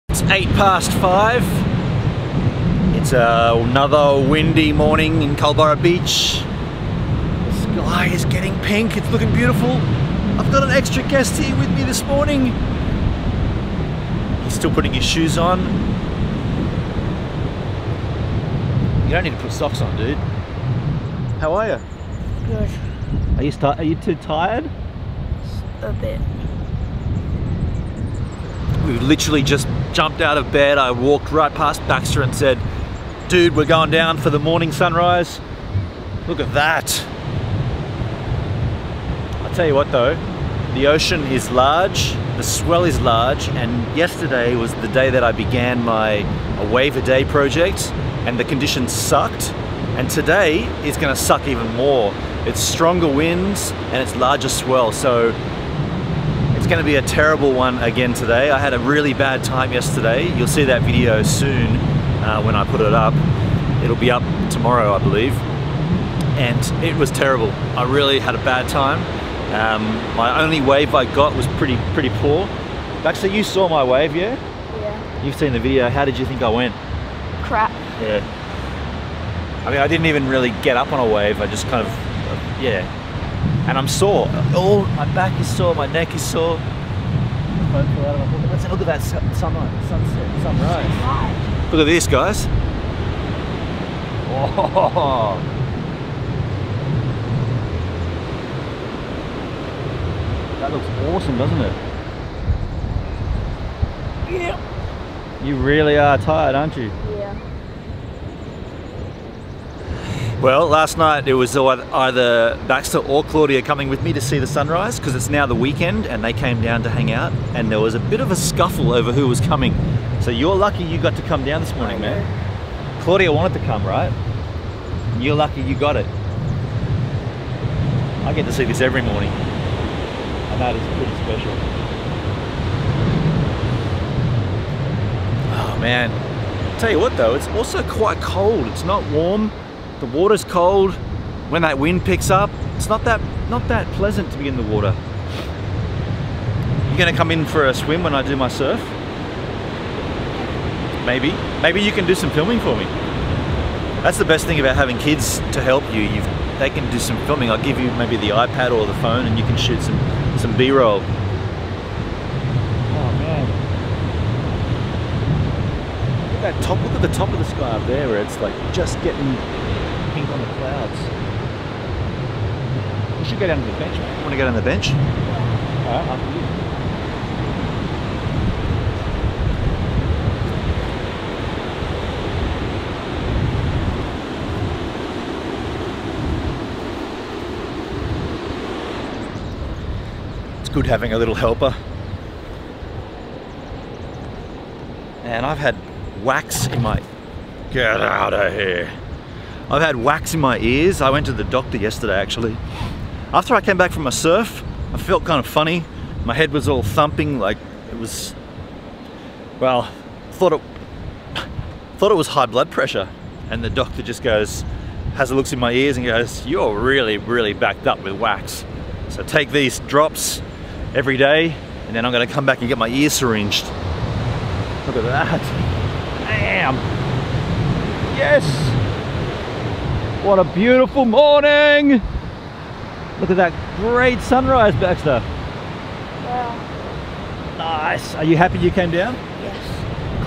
It Was Too Early For Him
Baxter came and joined me on my daily vlog this morning but it was too early for him! If you're a photographer you may find my Lightroom Presets handy

This is daily vlog 438 since I began this channel in August 2015 and today between 05:08 and 05:12 I was at home in Culburra Beach with Baxter talking about how sore I am from my A Wave A Day project yesterday.